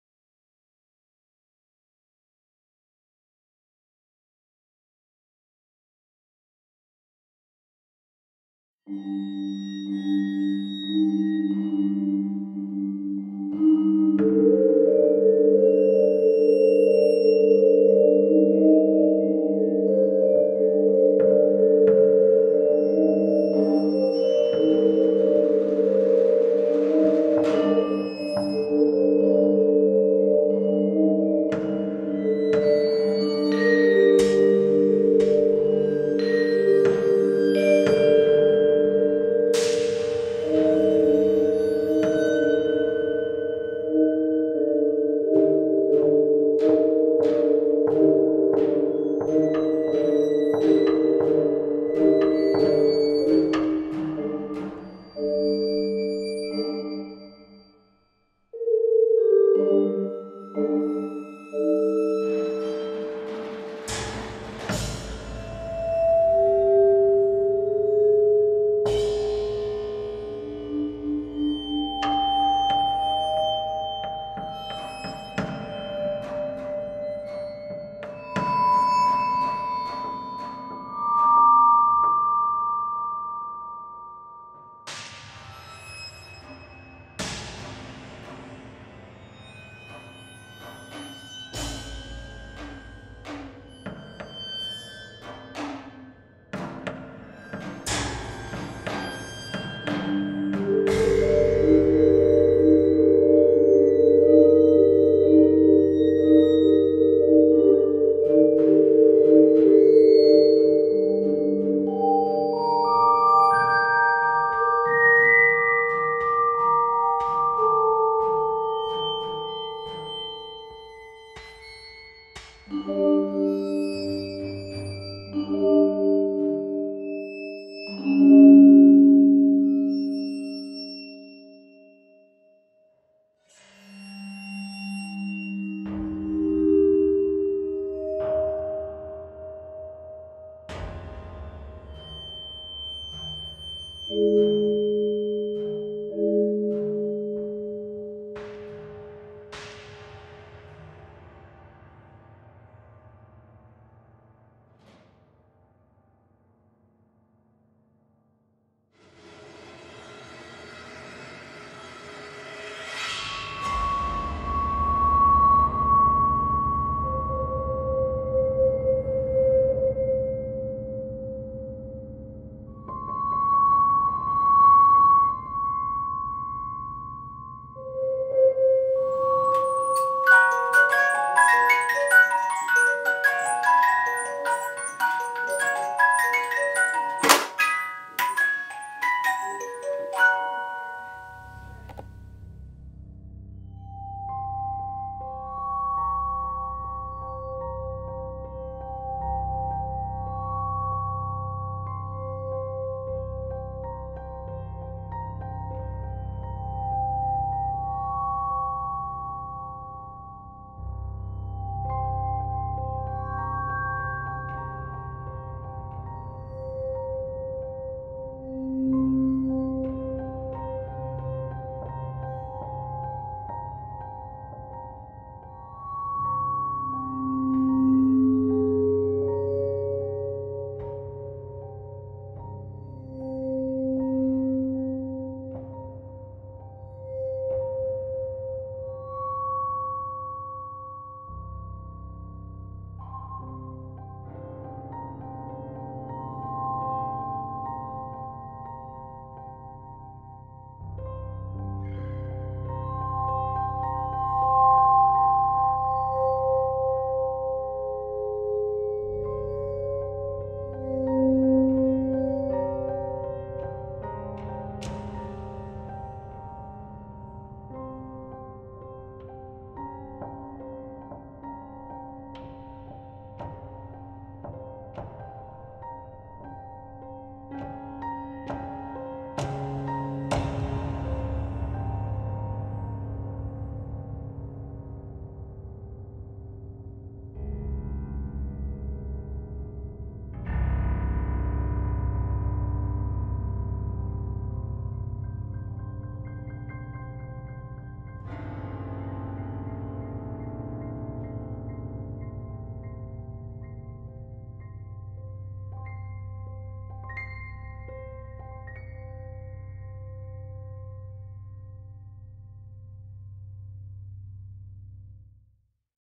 the clock broke. | AVANT-GARDE
Time stopped working the second we entered quarantine.
An avant-garde short film, made for the SCRN119 final.